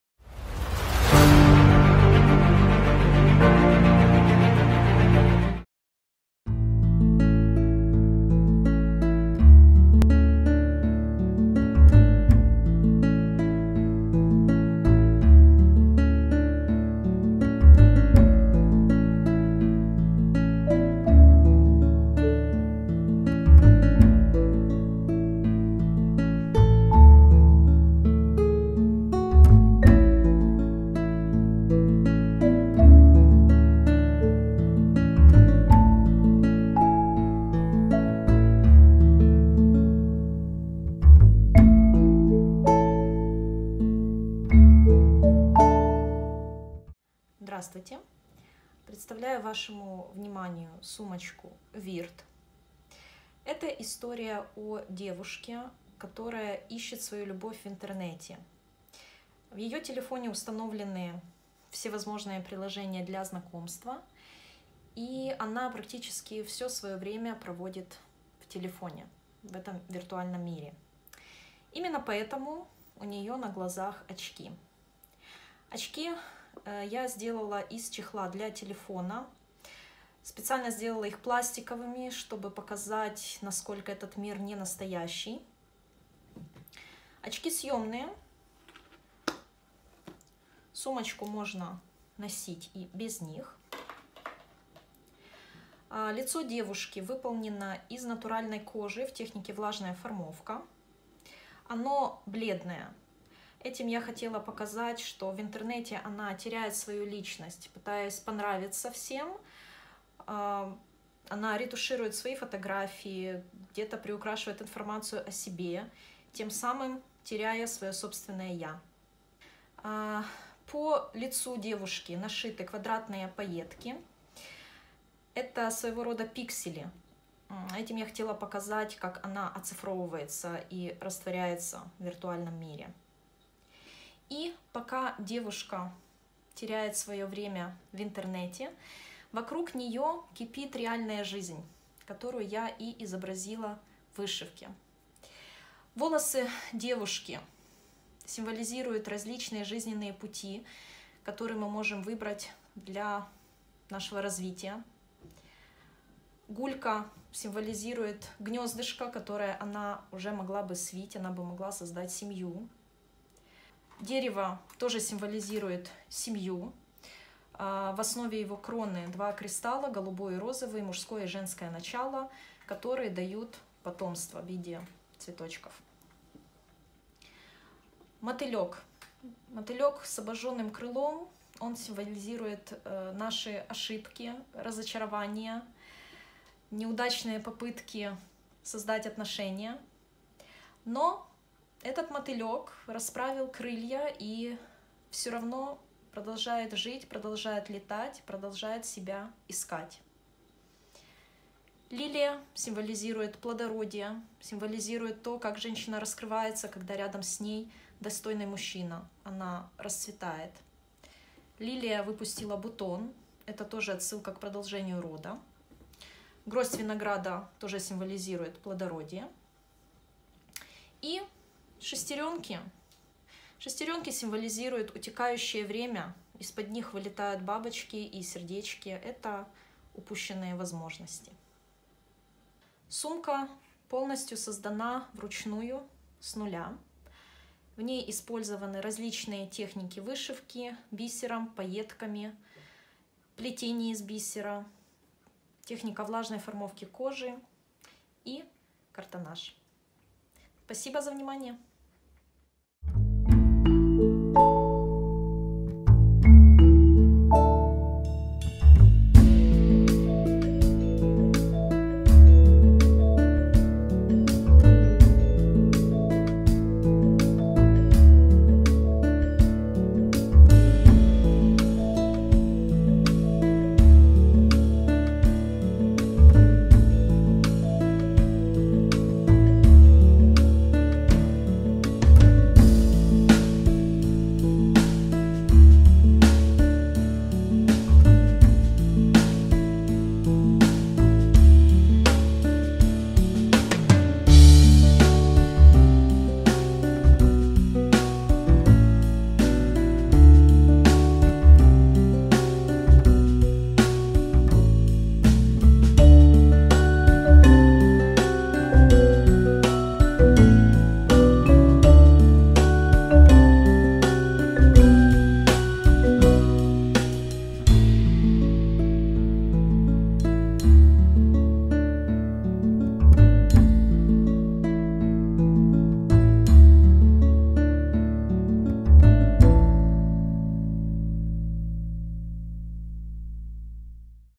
Golden Time Distant Festival | 17 Season | Nataliia Steblina | GTPS-1701-1267
Имя участника или коллектив: Nataliia Steblina

Страна: Ukraine

Название конкурсной работы: Сумка "VIRT"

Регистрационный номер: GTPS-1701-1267

Номинация: Дизайн

17 Season

Участие в других конкурсах и фестивалях / творческие достижения:
— Многократный победитель конкурсов Swarovski
— Приглашенный судья летнего конкурса Swarovski
— Финалист конкурса Bead Dreams 2019
— Приглашенный спикер украинских курсов
— Автор обучающих МК
— Основатель киевского клуба бисерозависимых